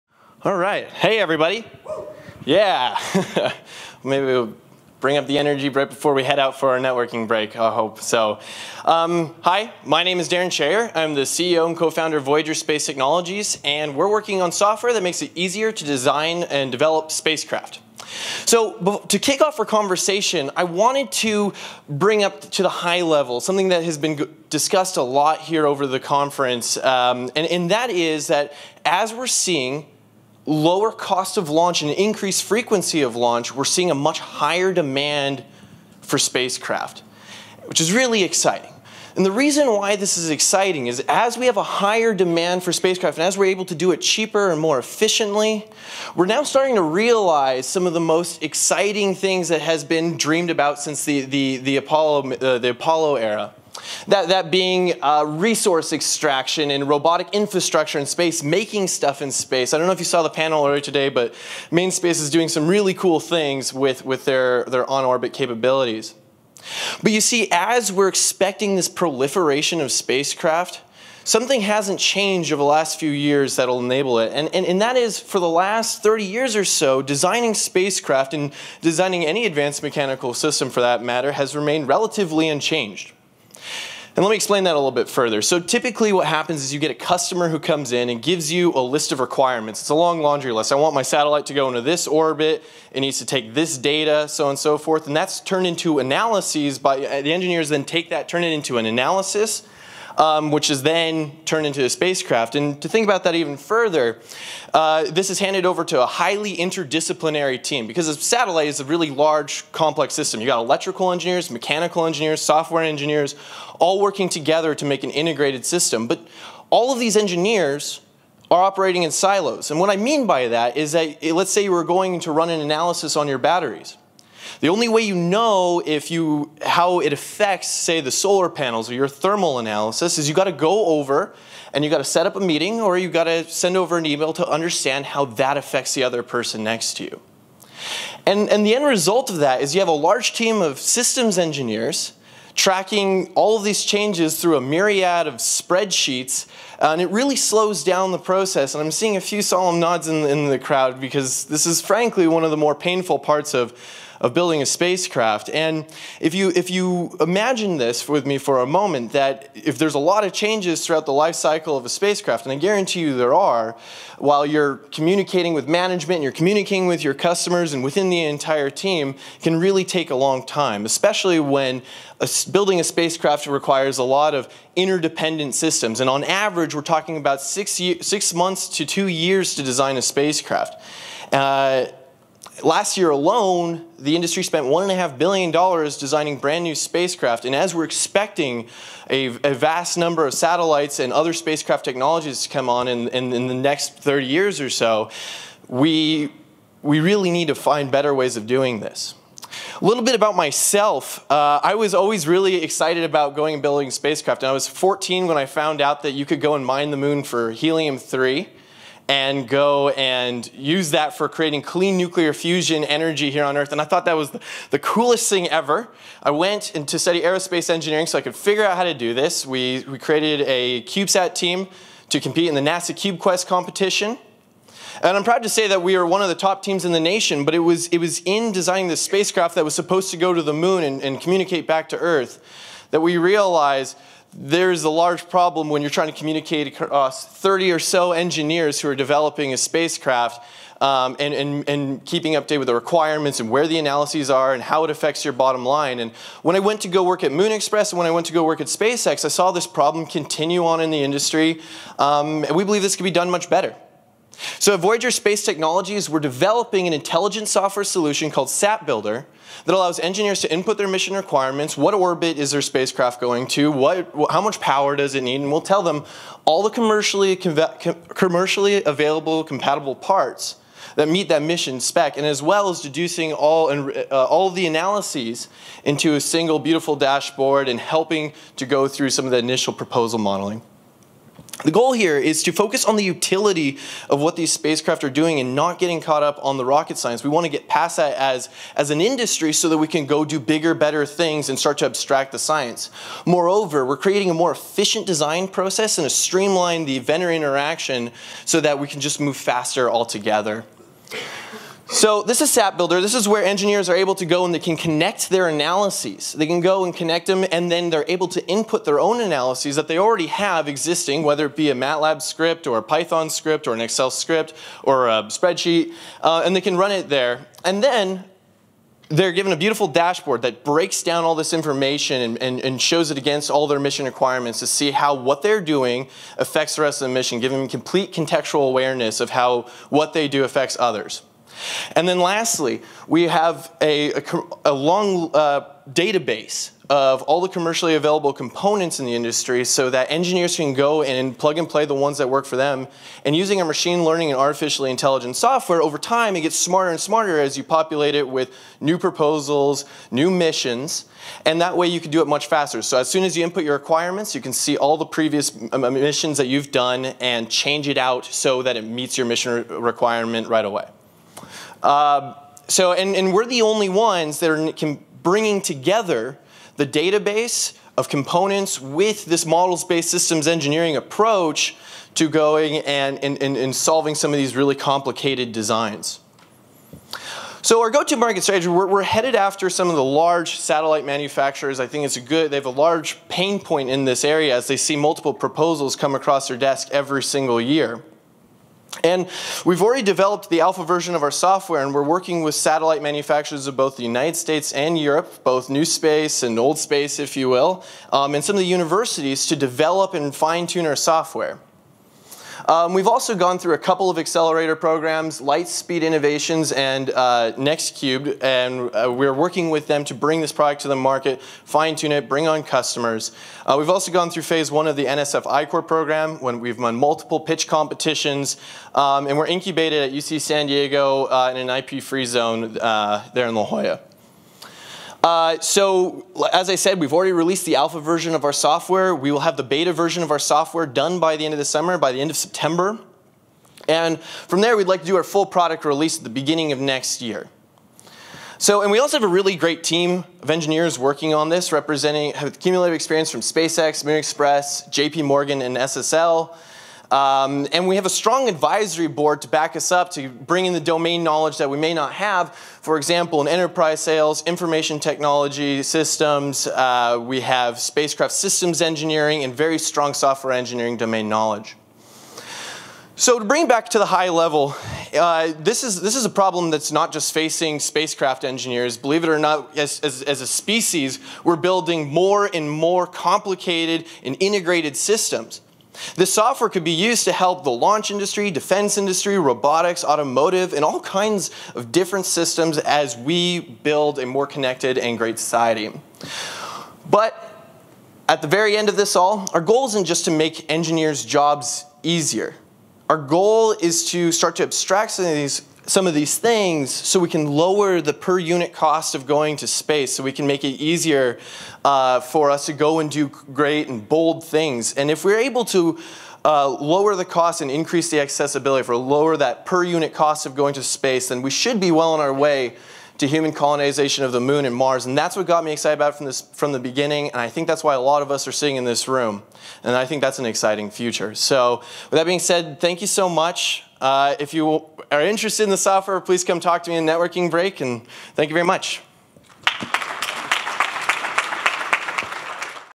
NewSpace 2018: Quickfire Talk - Voyager
Darren Charrier, CEO and Co-Founder of Voyager